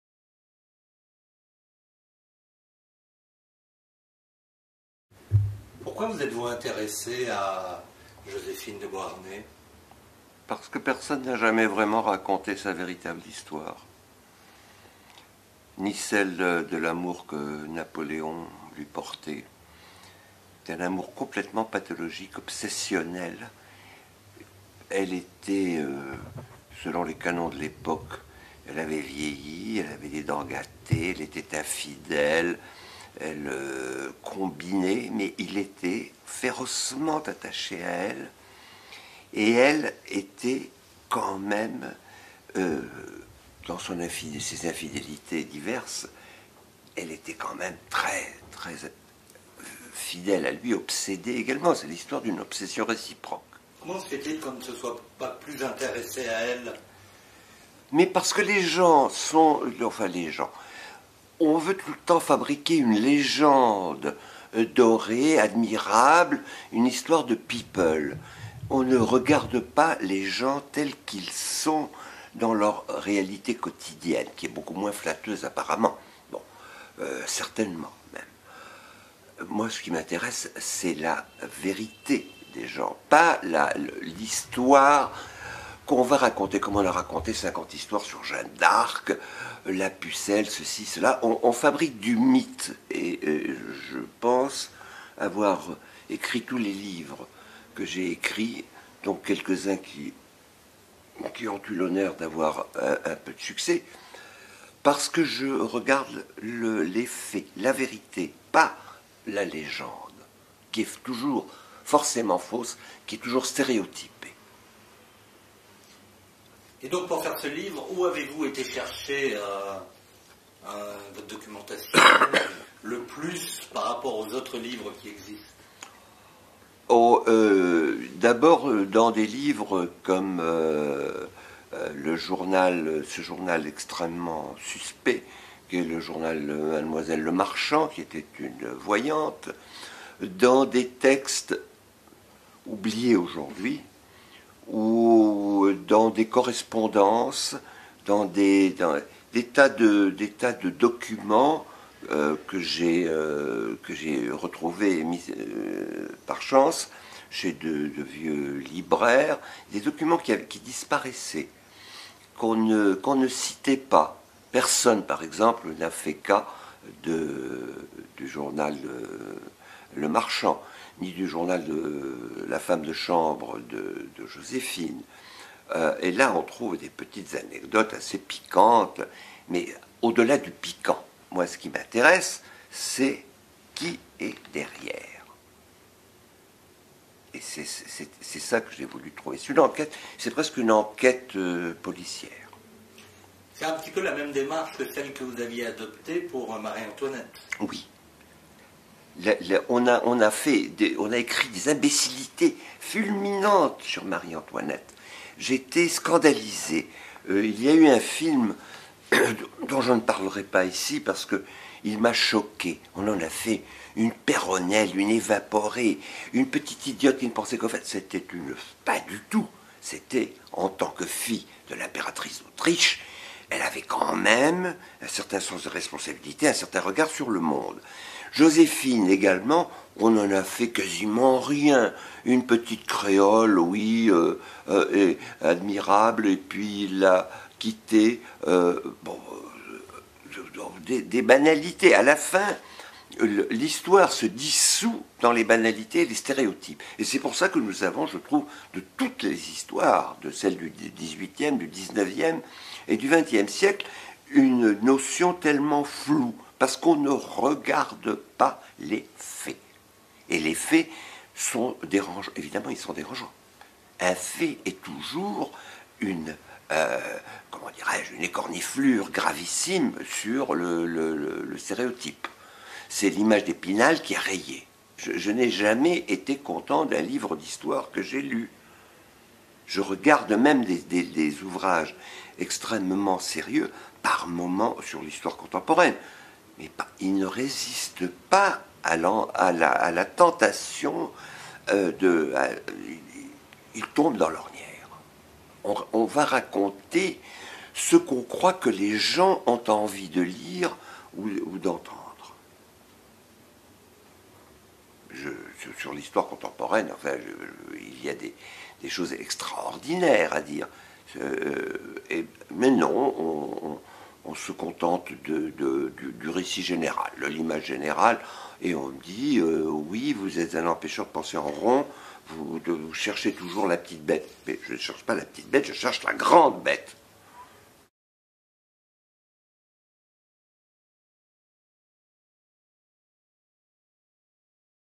Gerald Messadié
Gerald Messadié évoque les amours fusionnelles, obsessionnelles et tumultueuses de Napoléon et Joséphine.
"Joséphine, l'obsession de Napoléon", 400 p., 22 €.
En librairies le 16 mars 2011.